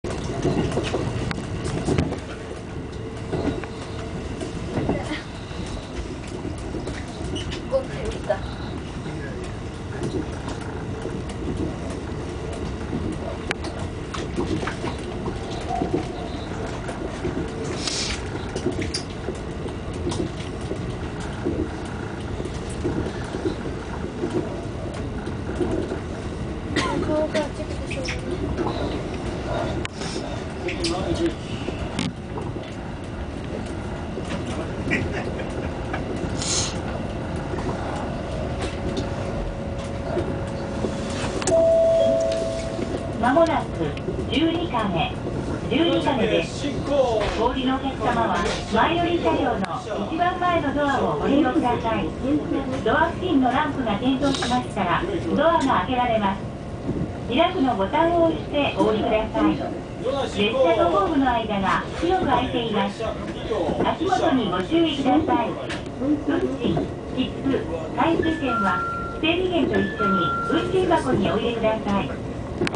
Visão de frente da cabine do maquinista (18)
Bom em muitos trens japoneses, no primeiro vagão, dá pra ter a mesma visão que o maquinista tem...mó louco!